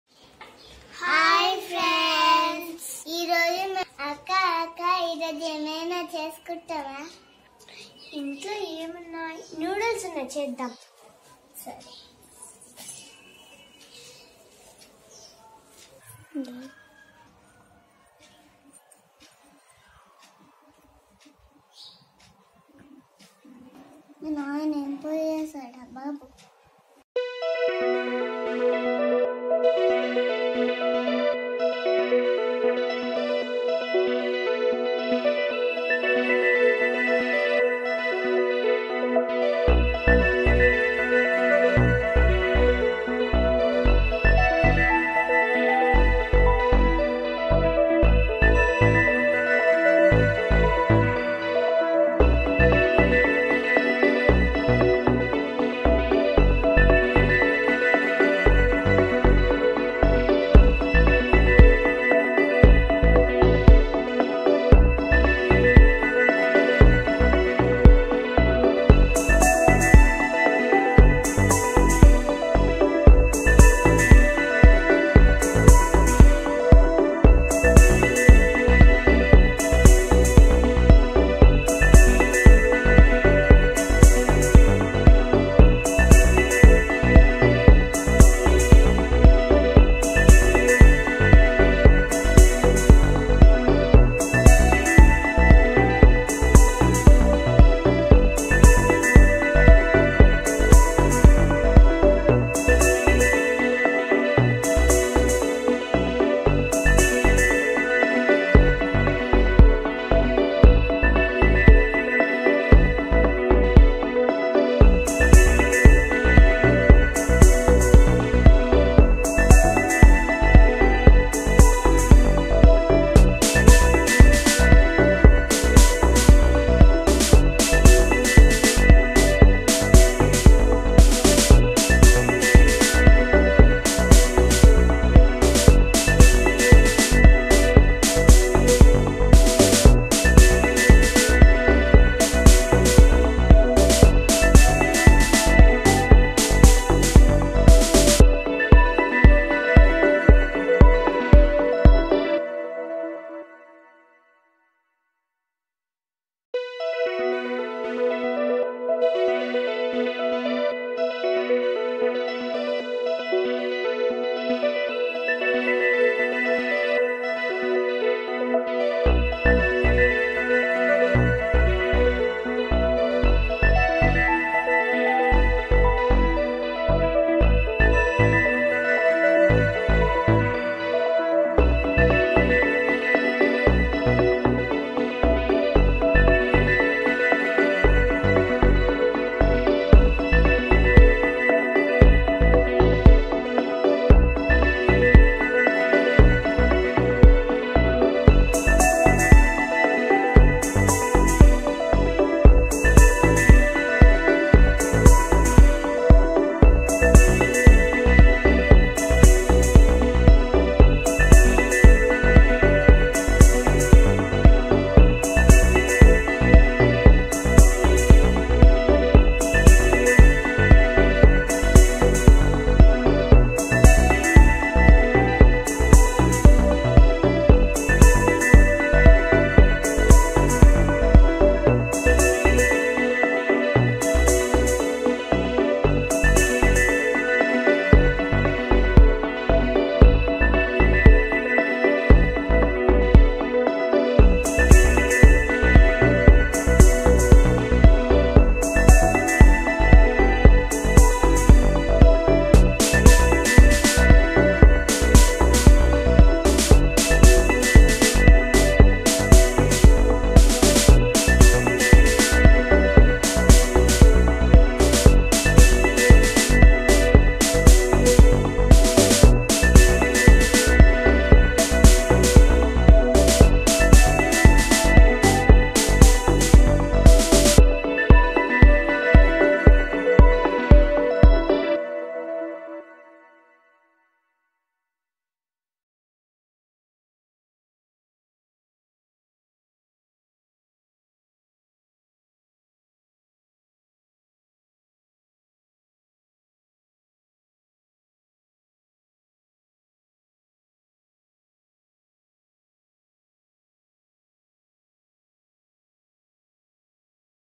Miniature egg resipi | egg noodles omlette |mini food|kids cooking
Welcome to kids cooking Mini food | in this video we are cooking funny Miniature noodles| day real mini foods this food are like tha kids mini food . Tasty noodles indoor kids cooking.you also cook your in Howes

Enjoy Mini foods  videos videos

Miniature samosa